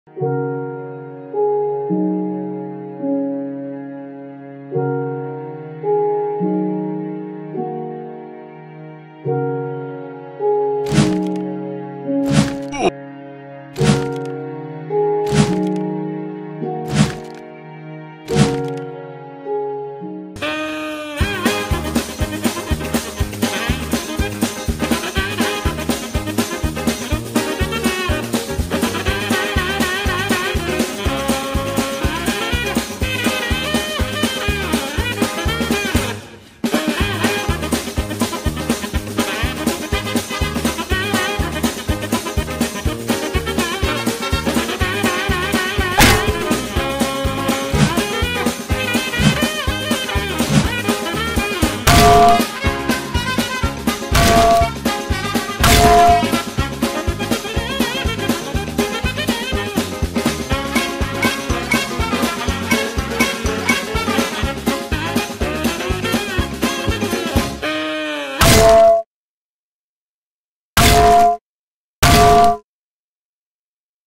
Getting my first pan kill took a lot more running than I thought...
The source video have no sound so I have to add all the sound effects myself..